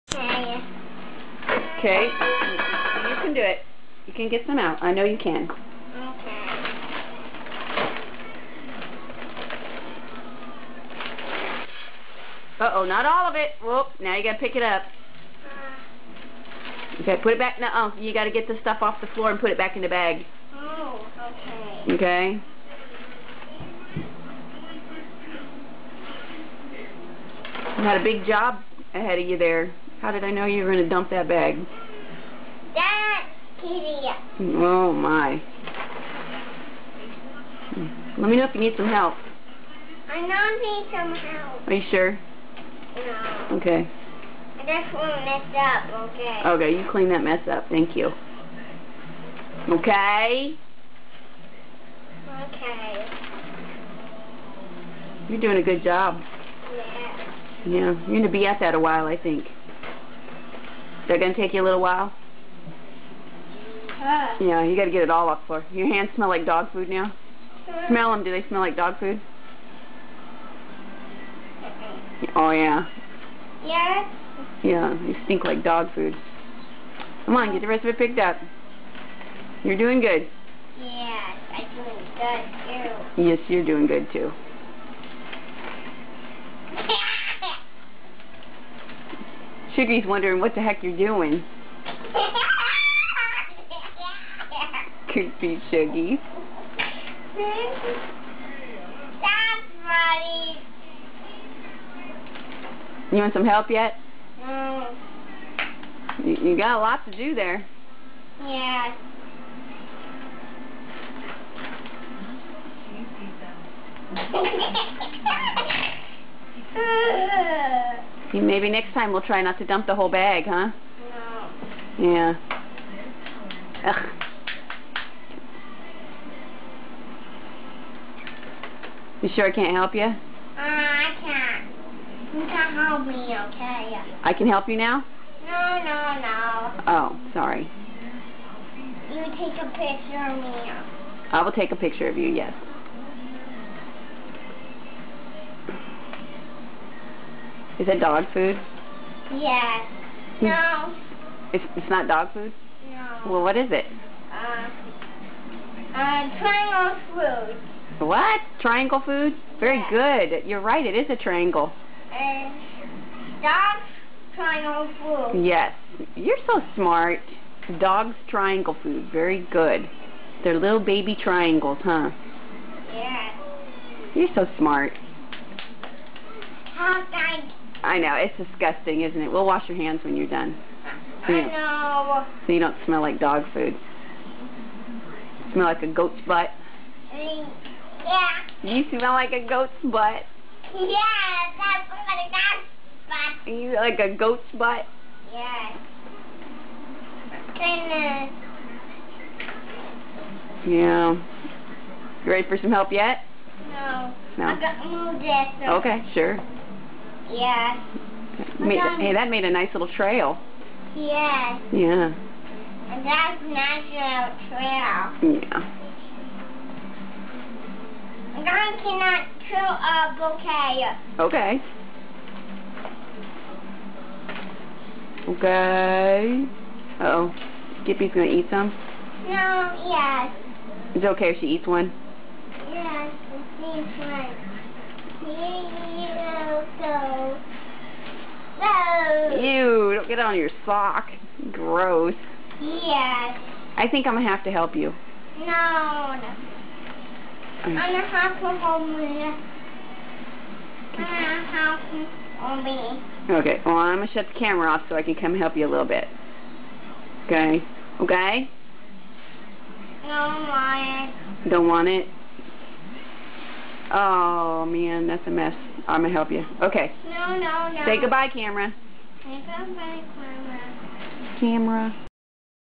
Bubba Pouring Dogfood
Its so hard to be patient and calm when your toddler is trying his hardest to help you.